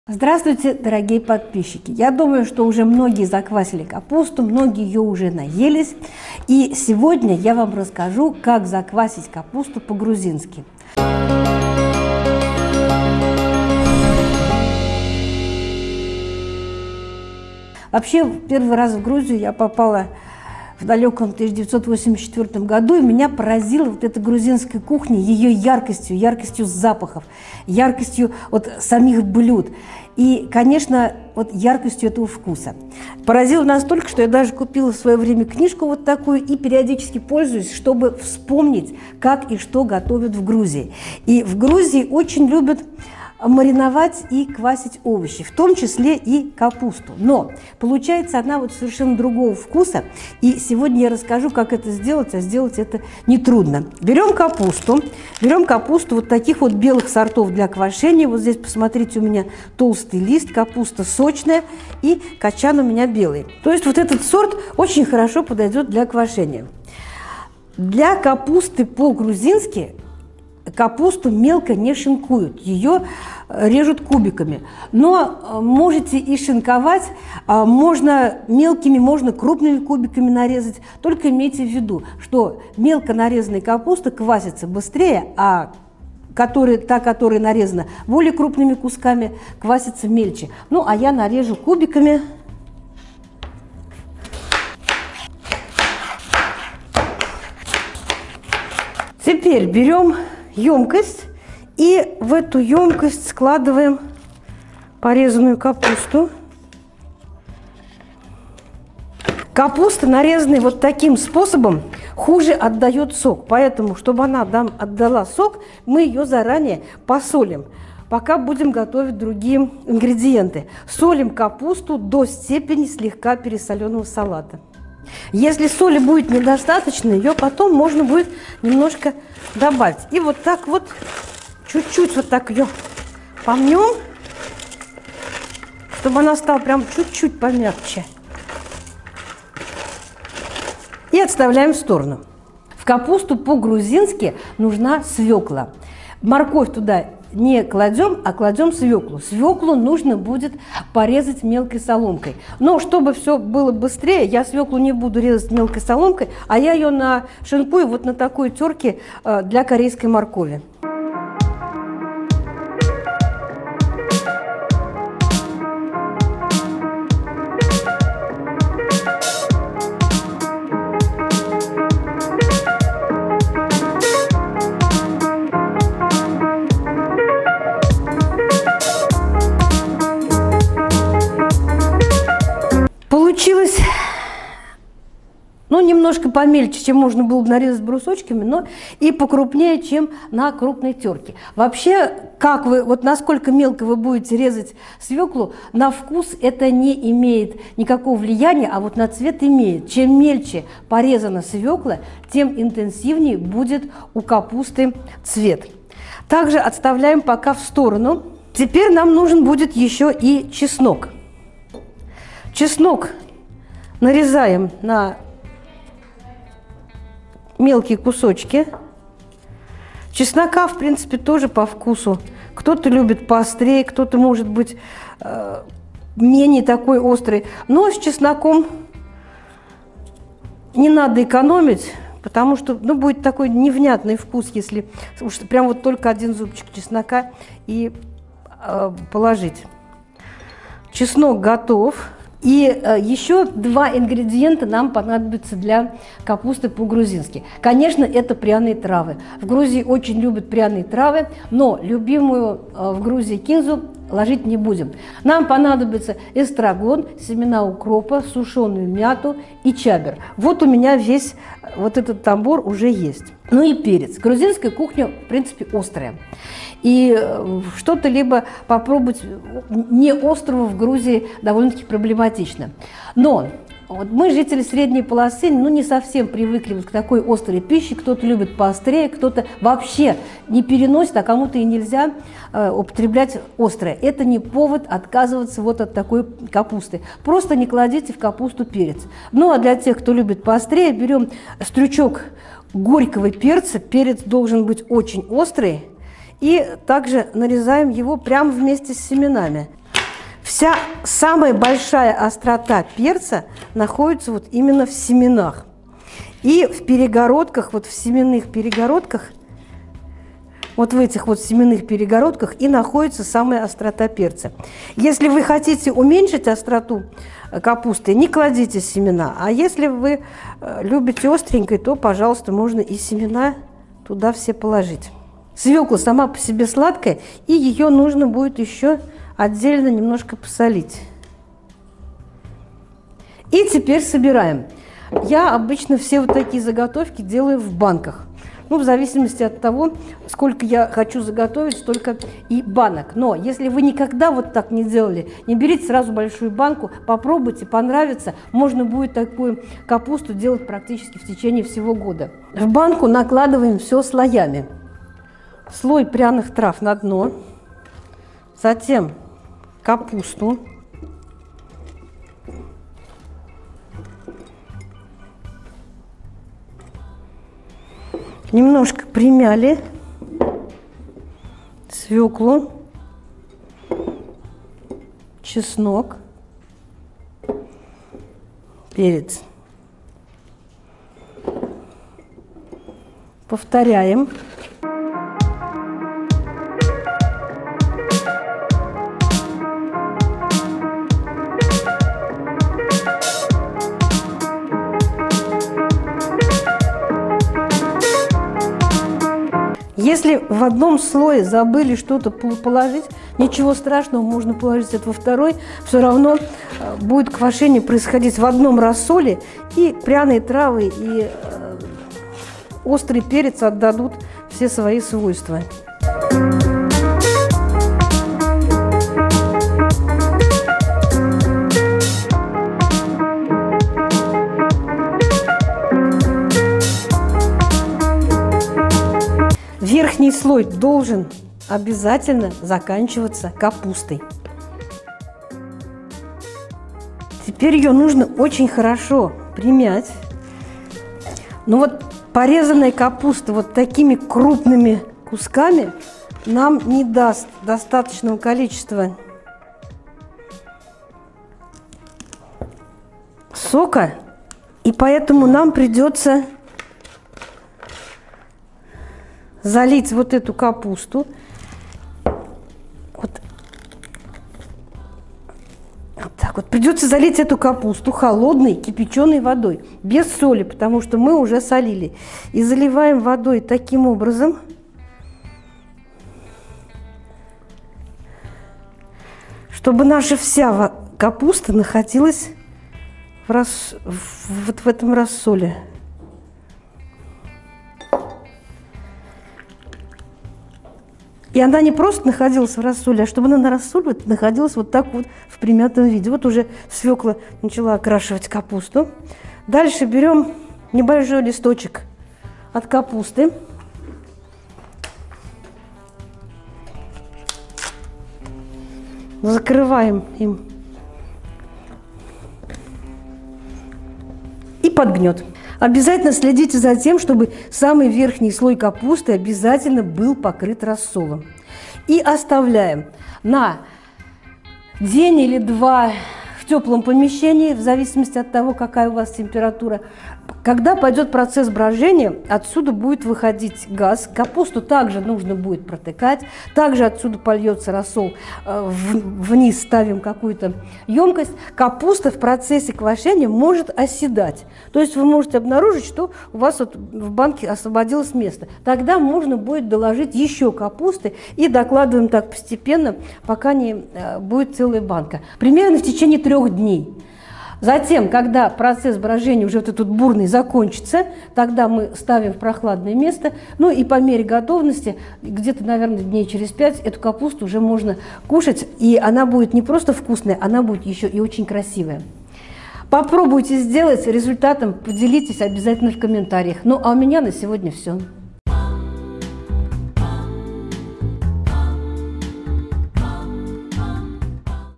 ВКУСНАЯ КАПУСТА ПО ГРУЗИНСКИ ✔️ Острая Квашенная Капуста ✔️ Рецепты от Garden-zoo
Квашеная капуста по- грузински.

Грузинская кухня яркая по запаху, вкусу и цвету.
Для квашеной капусты по-грузински нам понадобится капуста, свекла, чеснок, смесь пряных трав, соль, острый перец.
Капусту режем квадратами, свеклу соломкой (можно натереть на терке для моркови по-корейски), чеснок и перец мелко режем.
В банку выкладываем слоями: капусту, пряности, свеклу, перец. Если сока мало – доливаем холодной кипяченой воды.
На 1кг капусты нам нужно: 300-400гр интенсивно окрашенной свеклы, 150-200гр сельдерея с кореньями, сушеной зелени чабера, базилика, эстрагона, мяты, семена укропа, 100-150гр чеснока, 2 столовые ложки соли, острый перец по вкусу.

Товары представленные в роликах вы можете приобрести по адресу:
г. Пенза, ул.Суворова 225, магазин "Зеленый дом"